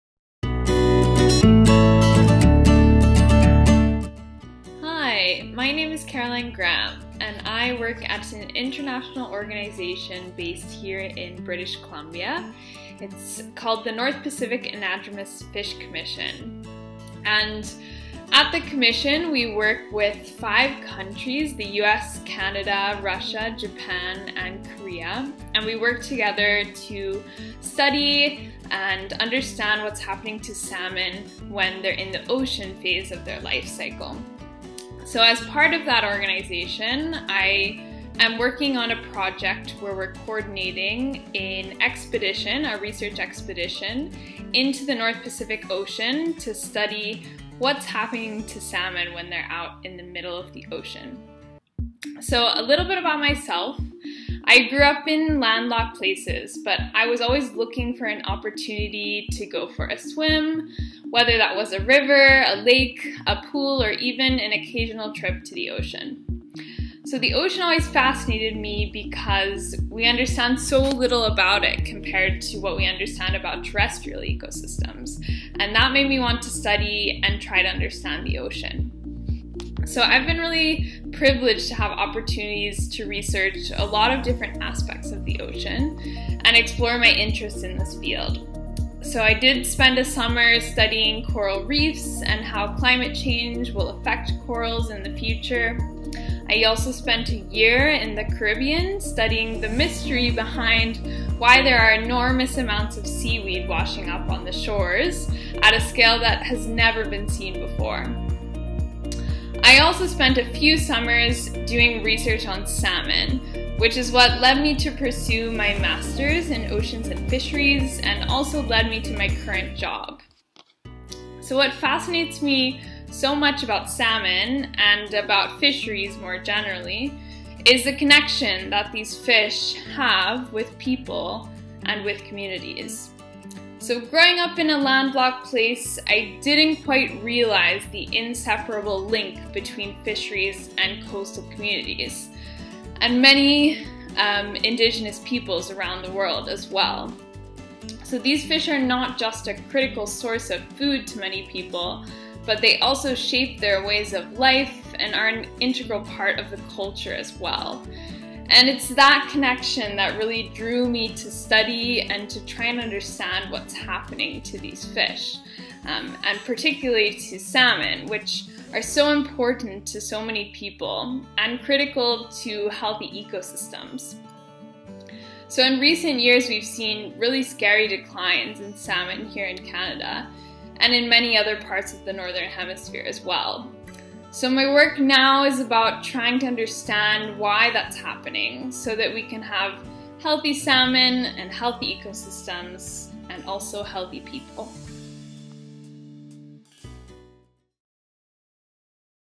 Caroline Graham Mentor Video
Mentor highlight video from Life Below Water Online Course for grade 8-12 students. This course was created by Sea Smart in collaboration with the Society for Canadian Women In Science and Technology. Sign up for the course at: